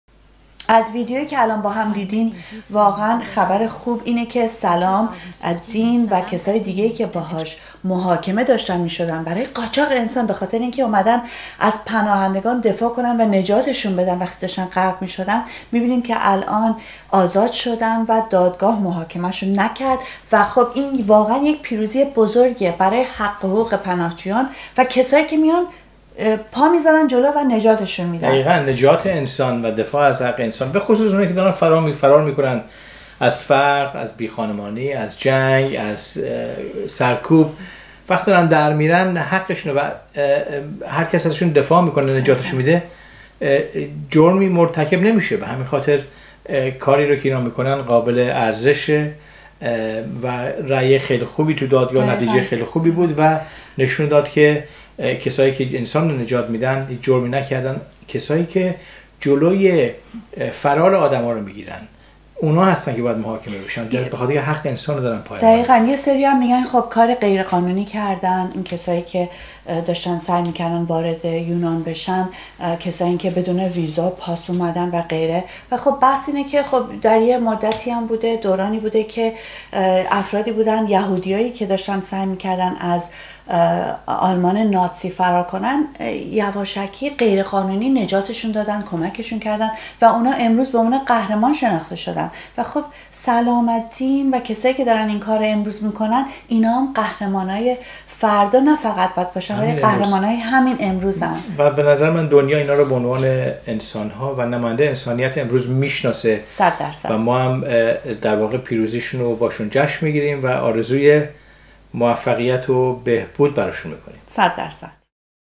لحظه زيباى زندگى: سلام الدين ـ قاچاق انسان يا نجات پناهجويان؟
لحظه زيباى زندگى: سلام الدين ـ قاچاق يا نجات پناهجويان؟
صد در صد نجات انسان
زنده باد
SUPPORT BREAD AND ROSES TV WITH AS LITTLE AS $1 A WEEK: